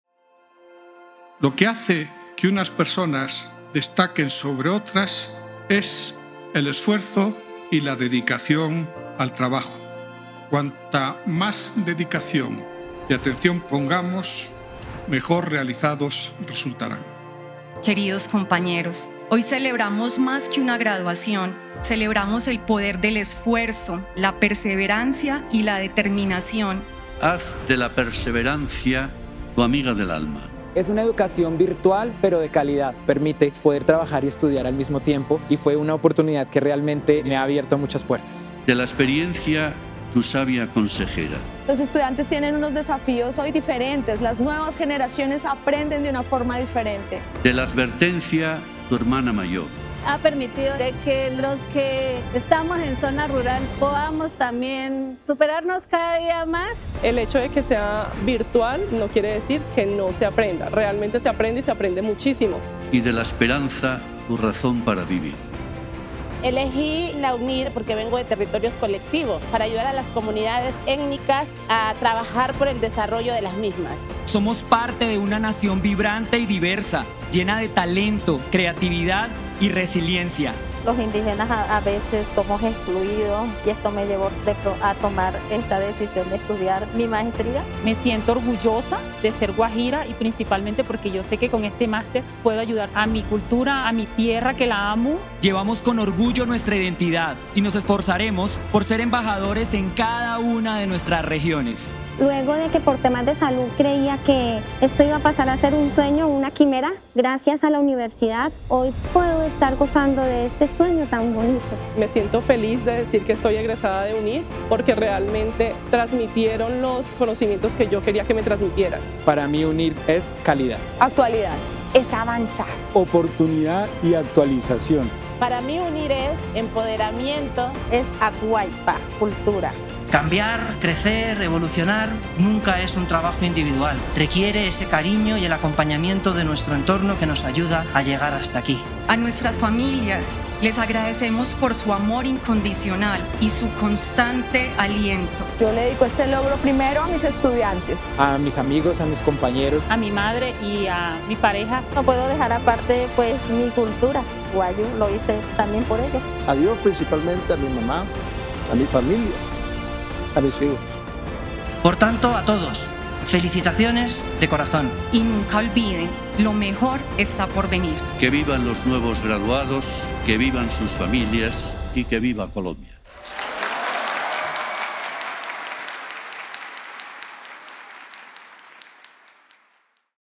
Así vivimos nuestra Graduación UNIR Colombia 2024
Los días 17 y 18 de mayo celebramos en Bogotá nuestra Graduación UNIR 2024, donde otorgamos los títulos universitarios a cerca de 1.700 egresados que han completado su Maestría de manera exitosa.
Nuestro gran motor son nuestros alumnos y nos sentimos orgullosos de cada uno de ellos que han logrado conseguir su título y compartimos su éxito. Sus historias, son nuestras historias. ¡Felicidades graduados!

“Para UNIR el estudiante ocupa el centro de su actividad docente y la graduación supone el premio a un recorrido en el que ha invertido esfuerzo en su desarrollo personal y profesional”, José María Vázquez García-Peñuela, rector de UNIR

¡Conviértete en nuestro próximo egresado de una Maestría de Calidad Europea!